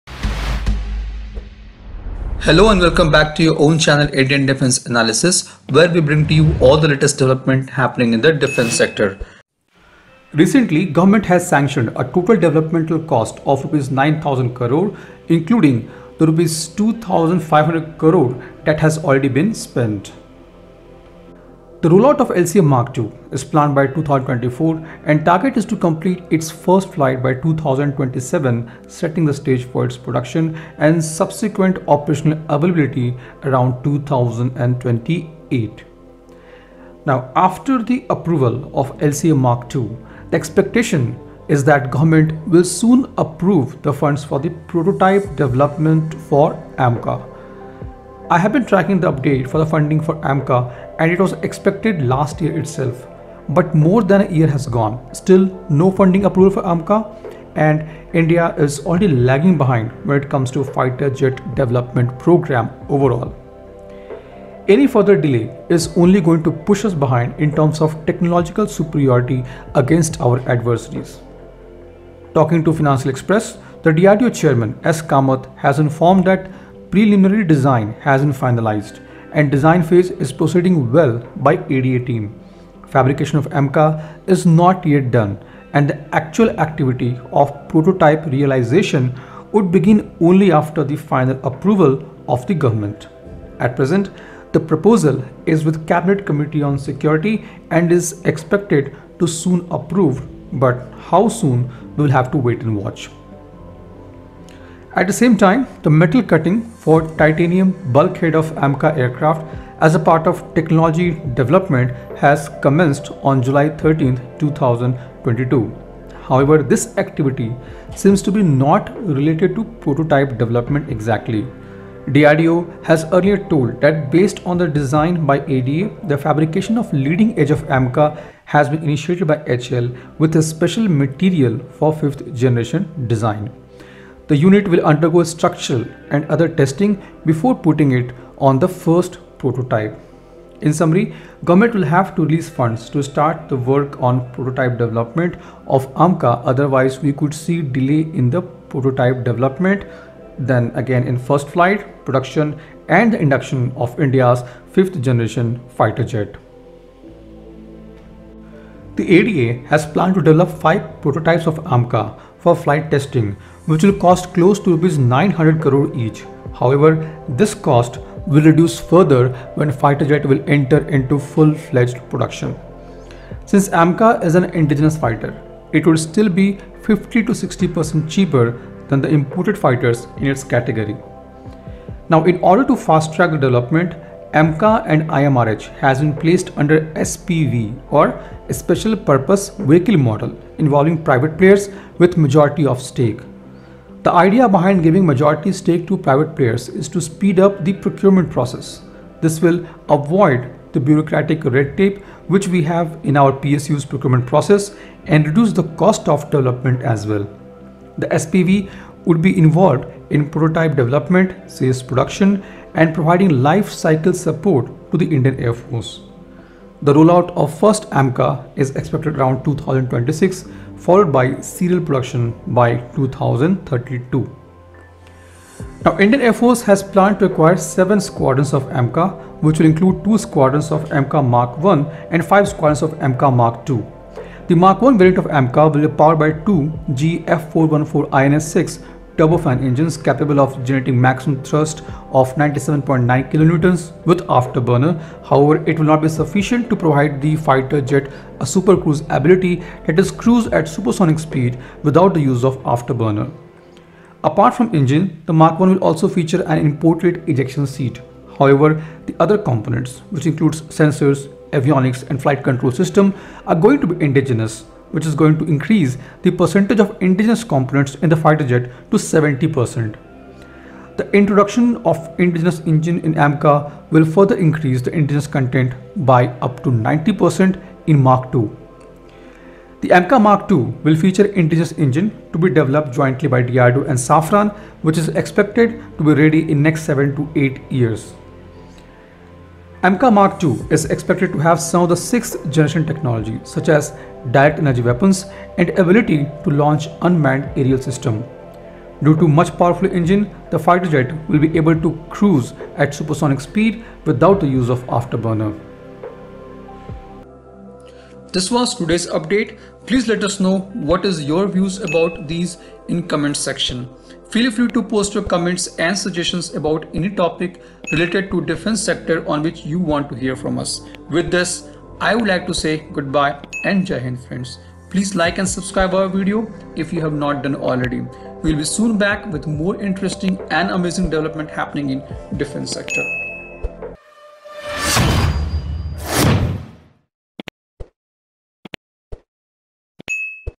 AMCA : India's 5th generation fighter jet | Awaits fund for prototype development
ADA awaits fund from govt. for AMCA prototype

Recently, government has sanctioned a total development cost of ₹9000 crore including the ₹2500 crore that has already been spent. The roll out of LCA-Mk2 is planned by 2024 and the target is to complete flight testing by 2027, setting the stage for its production and subsequent operational availability around 2028. After the approval for LCA Mark2, the expectation is that government will soon be approving the funds for the prototype development of AMCA. I have been tracking the update for the funding for AMCA and it was expected last year itself. But more than a year has gone still no funding approval for AMCA. India is already lagging behind when it comes to fighter jet development program. Any further delay is only going to push us behind in terms of technological superiority against our adversaries. Talking to financial express the DRDO Chairman S. Kamat has informed that the Preliminary Design has been finalized and design phase is proceeding well by ADA Team. Fabrication of AMCA is not yet done and the actual activity of prototype realization would begin after final approval of the Government. At present the proposal is with CCS and is expected soon, how soon we will have to wait and watch.

Metal Cutting for Titanium Bulkhead of AMCA

At the same time the ‘Metal Cutting for Titanium Bulkhead of AMCA aircraft’ as part of technology development has started on July 13th 2022. However, this activity seems to be not related to prototype development. DRDO has earlier told that based on the design by ADA the fabrication of Leading edge of AMCA initiated at HAL with special material for 5th gen design. The unit will undergo structural & other testing before putting it on the first prototype.

In summary the government will have to release funds to start the work on prototype development of AMCA otherwise, we could again see delay in the prototype development, first flight, production and induction of the India’s 5th generation fighter jet.

Development: 5 prototypes

The ADA has planned to develop 5 prototypes of AMCA for flight testing which will cost close to Rs 900 crore each. However, this cost will reduce further when the fighter jet will enter into the full fledge production. Since AMCA is an indigenous fighter, it would be still 50-60% cheaper than the imported fighters in its category.

In order to fastrack the development, AMCA and IMRH has been placed under SPV or Special Purpose Vehicle Model involving private players with majority of stake. The idea behind giving majority stake to private players is to speed up the procurement process. This will avoid the bureaucratic red tape which we have in our PSUs procurement process and reduce cost of development as well. The SPV would be involved in the prototype development, series production and providing lifetime/cycle support to the Indian Air Force.

The rollout of first AMCA is expected around 2026 and serial production by 2032.

Variants : Mark-I & Mark-II

Indian Air force has planned to acquire 7 Squadrons of AMCA which will include 2 squadrons of AMCA Mark1 & 5 squadrons of AMCA Mark2.

The Mark1 variant of AMCA will be powered by two GE F414-INS6 turbofan engines capable of generating maximum thrust of 97.9KN with afterburner, however it will not be sufficient to provide the fighter super cruise ability i.e. cruise at supersonic speed without the use of after burner. Apart from Engine the Mark-I will also feature an imported ejection seat. However, the other components which includes sensors, avionics and flight control systems are going to be indigenous which is going to increase the percentage of indigenous components in the fighter to 70%. The introduction of Indigenous engine in AMCA will increase its indigenous content to 90% in Mark-2.